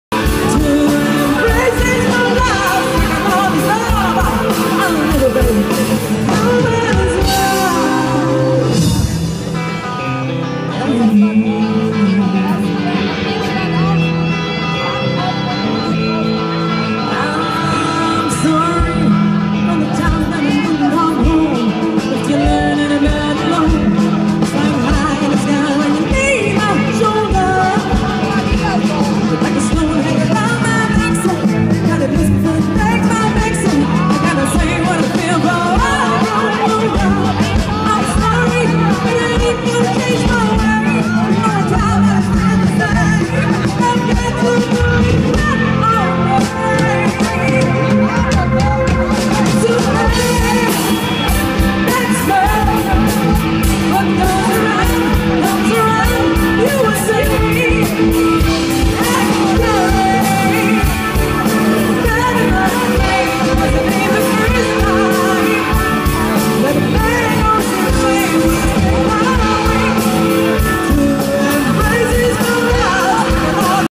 2014-09-27 Kerb Spachbrücken (2)
Ein paar Mitglieder des TC Gersprenztal Reinheim finden sich auf der Spachbrücker Kerb 2014 :)